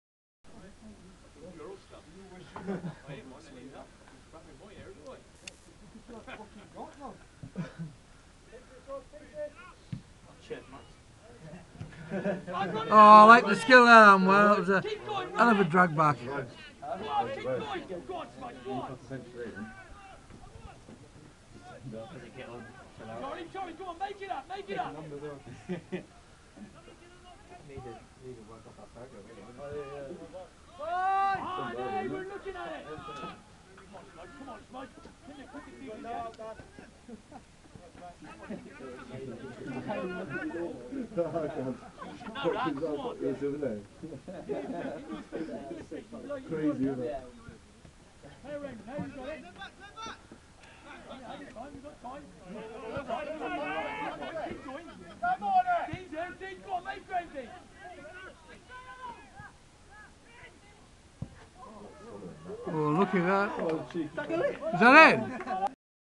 DUCHY Division 4 Game ,St Dennis Res 1 - 4 St Stephen Res
James Allen controls the pass from Dean Yelland and with the ball off the ground scores with this volley